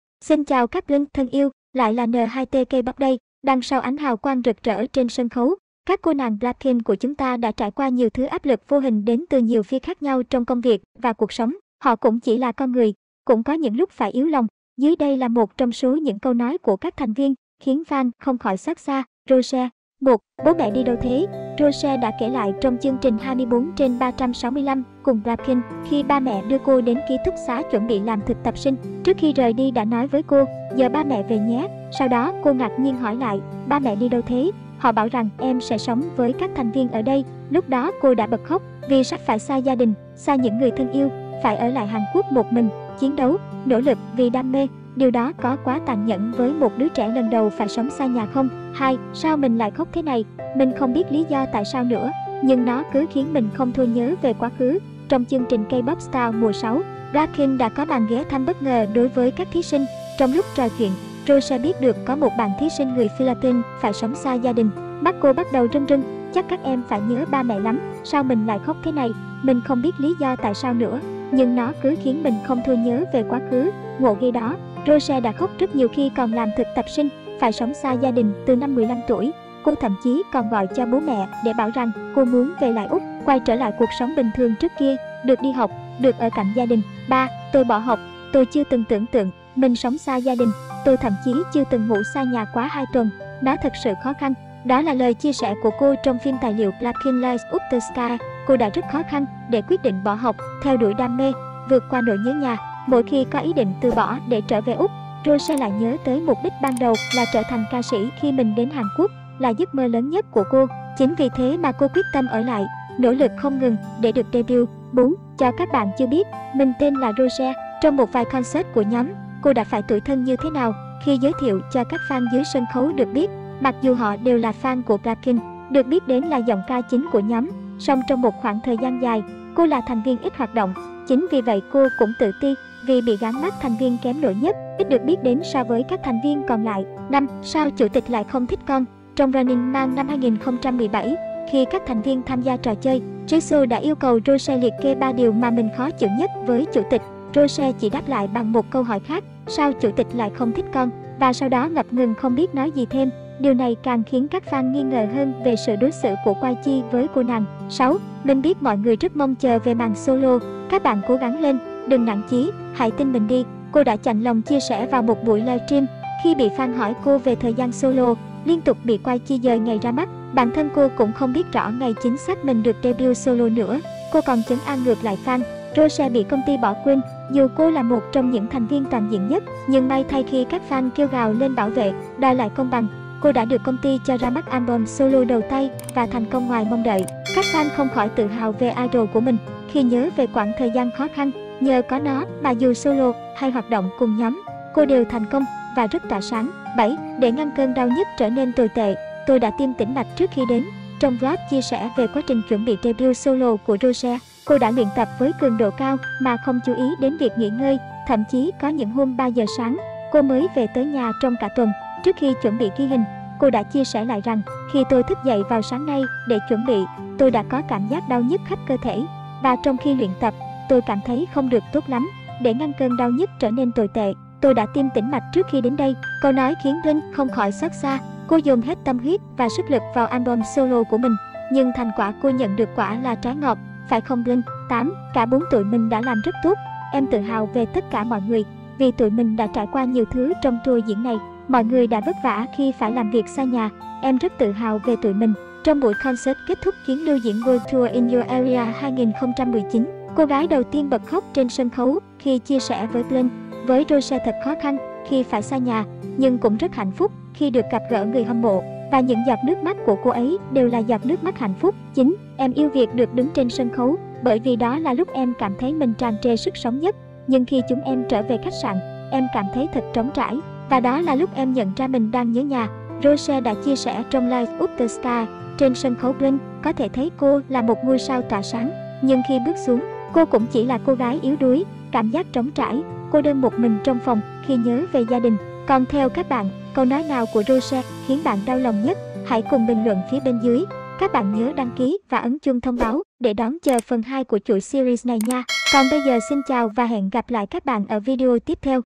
Những câu nói của Rosé (Blackpink) khiến fan không khỏi xót xa (Phần 1)
Kênh Thông Tin Giải Trí Tổng Hợp về Kpop (Blackpink)
★ Theo dõi N2T KPOP để cập nhật tin tức HOT nhất:
★ Video phát sóng vào lúc 20h vào các ngày trong tuần.